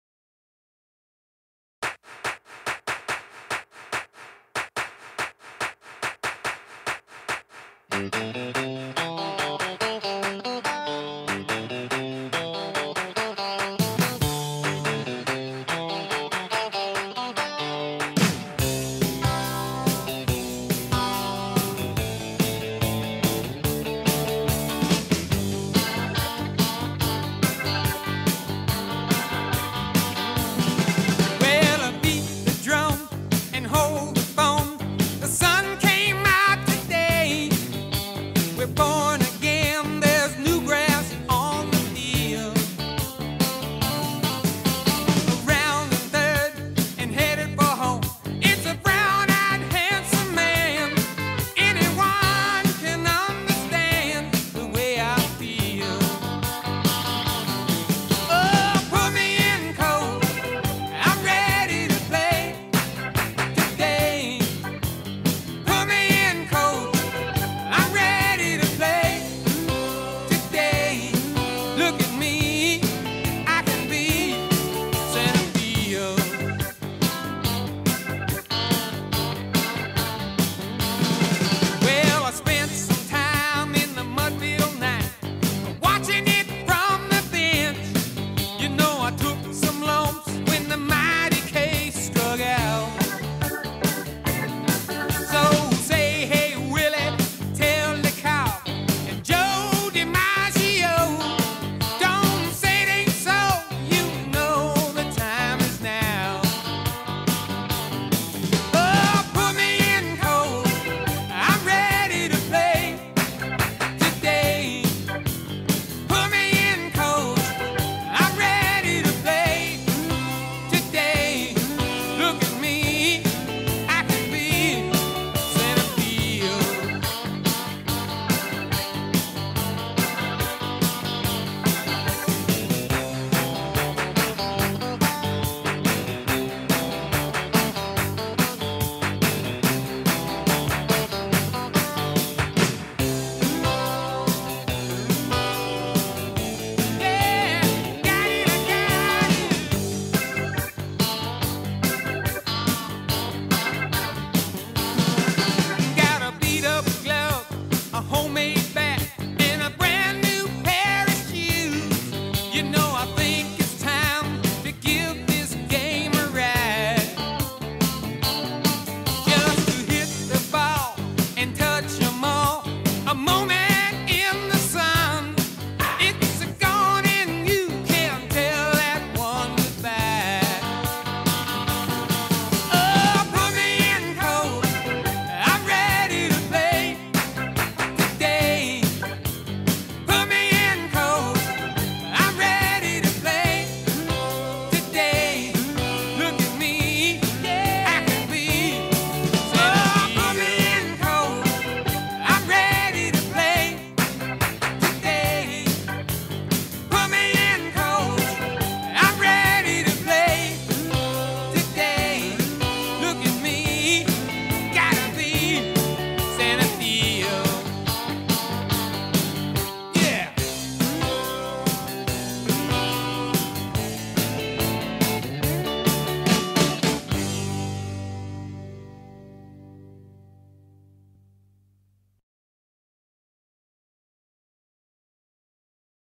LLIWS
Bill Nale (elivermore.com) and I take free photos of the Little League Intermediate World Series every year in Livermore, CA. We average about 40,000 photos in one week and upload them for the kids, no charge. Enjoy! More later if you like them.